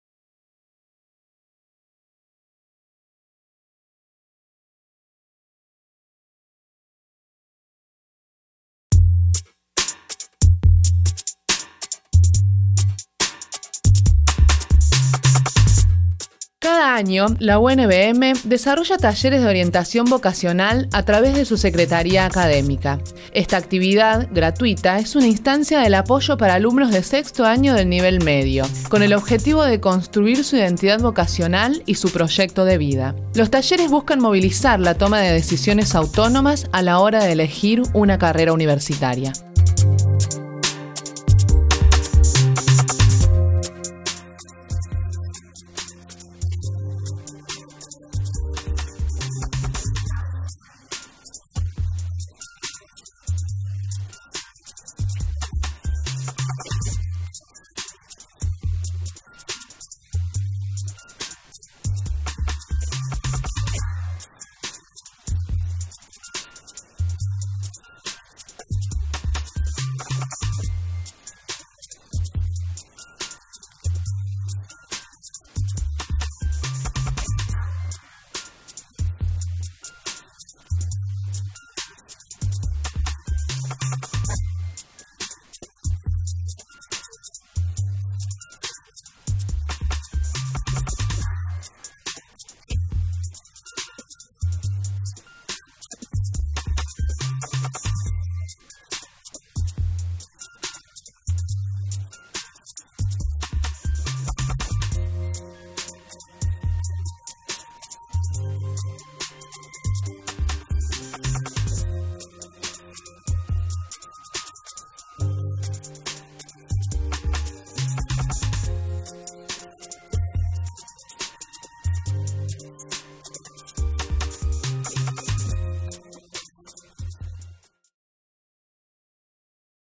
Talleres de Orientación Vocacional 2013
La actividad, dirigida a alumnos de sexto año, se constituye como instancia de apoyo para que los jóvenes construyan su identidad vocacional y su proyecto de vida. Iniciará el 9 de abril en el Campus.

Los talleres de Orientación Vocacional que impulsa la Universidad Nacional de Villa María son espacios gratuitos diseñados para que los alumnos del último año de Nivel Medio se sientan capaces y seguros de trazar su proyecto de vida, en relación al trabajo y/o estudio.